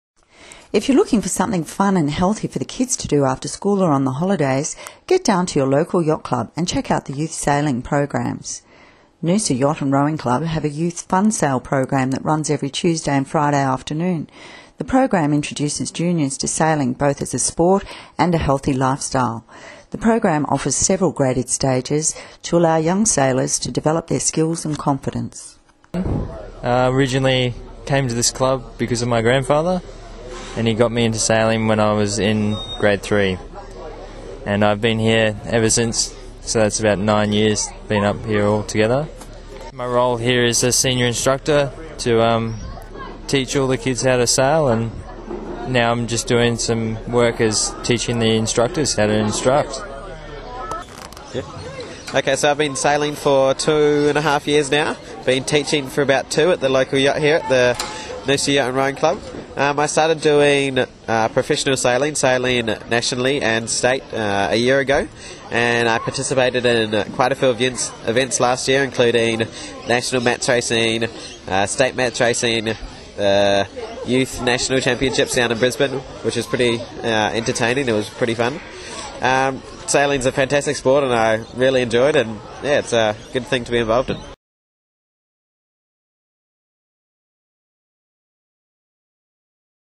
Coast kids sail through
Youth sailing programs offer kids the opportunity to participate in sport as well build confidence and learn new skills after school and on holidays.